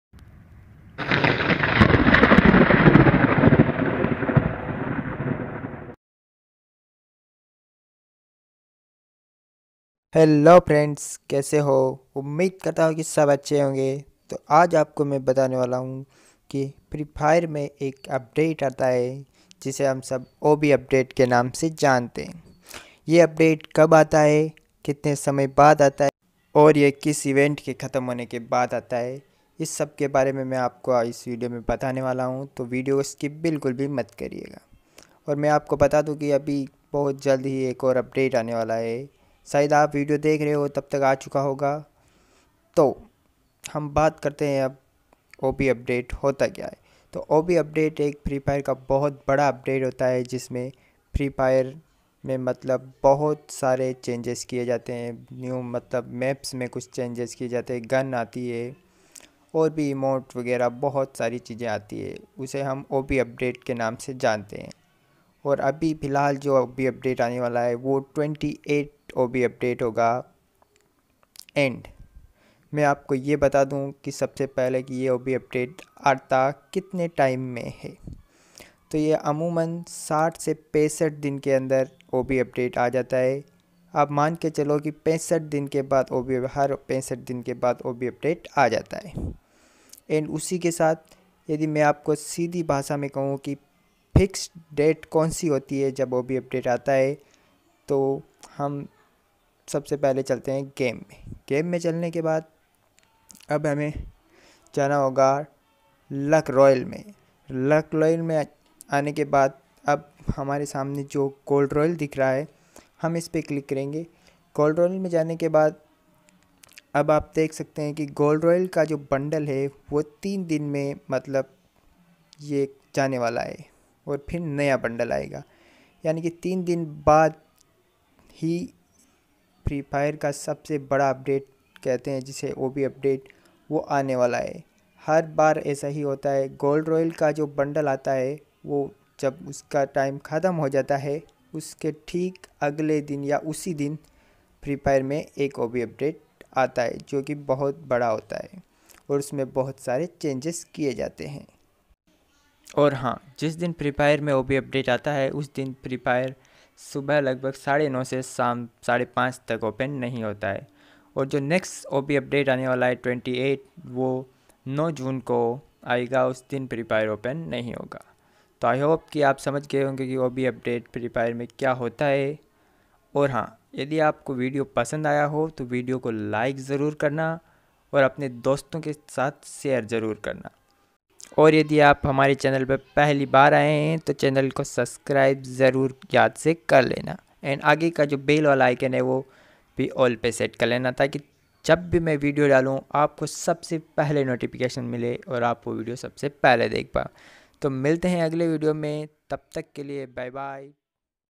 Free Fire OB Update Kab Aata Hai || FF OB UPDATE TIME || Garena Free Fire.
Hello Friends

Free Fire Id Name - ƁӇƛƦƛƬ__Bhai

Es video me mene free fire ke OB updates ke bare me btaya hai. Free fire OB update kitne time me aata hai. And OB update 28 date.

Or Ha

Ek bat or

Yedi aap bhi free fire me free diamonds lena chahte hai to.  Paytm First games. app jrur download kre.

My Paytm First games Code -  wpfsmz422